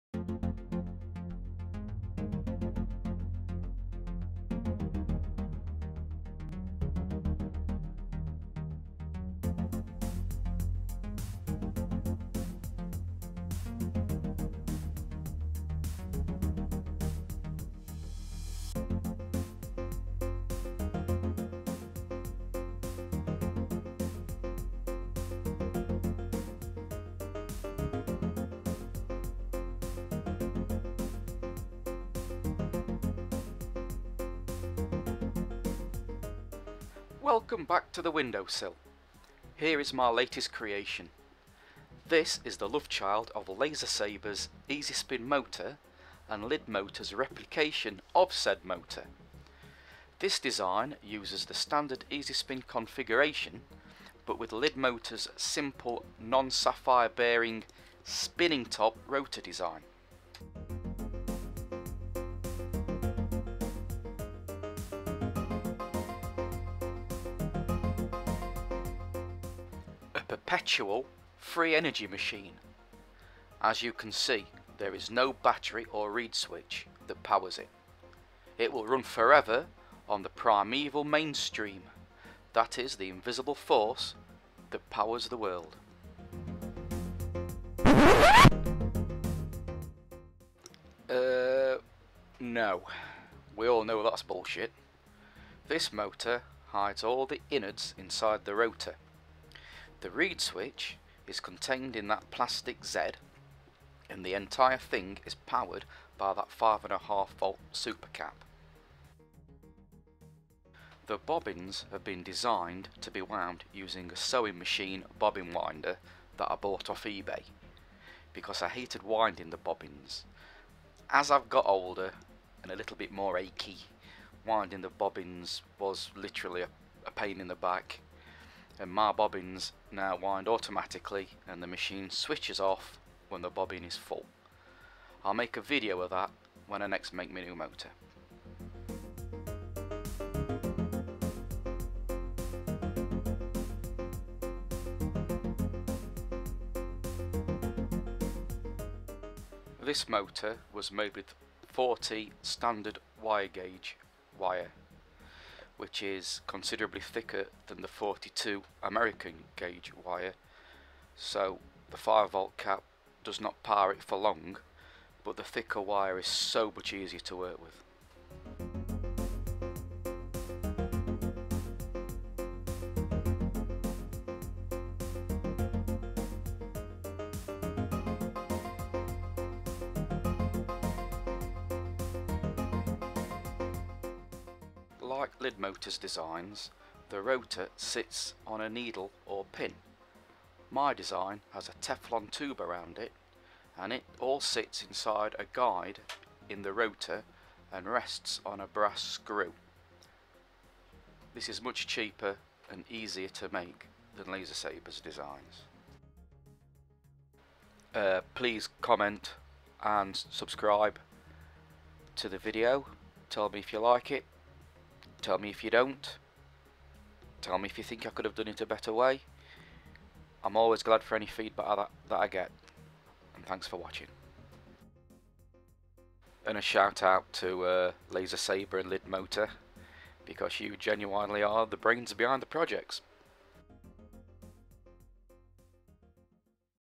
LaserHacker Lidmotor Child
This is a motor I desiged based totaly on LaserSaber and LidMotor's pulse motor designs.